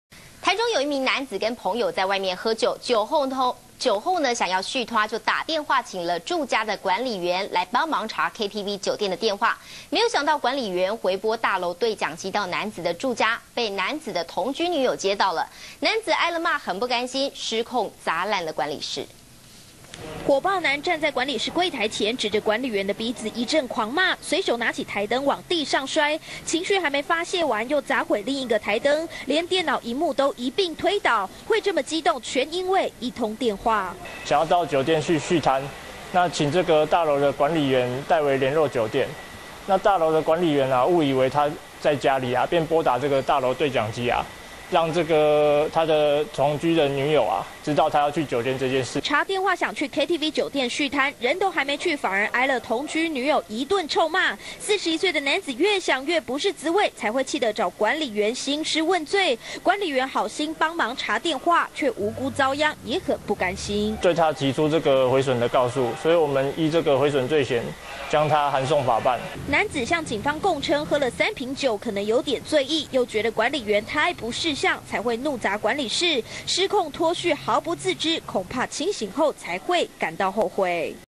中視新聞》上酒店被女友得知 男不滿醉鬧管理室
中視新聞》台中有一名男子日前和朋友在外喝酒，酒後想續攤，打電話請住家管理員幫忙查KTV酒店的電話，沒想到管理員回撥大樓對講機到男子住家，被男子的同居女友接到，男子挨了罵很不甘心，失控砸爛管理室。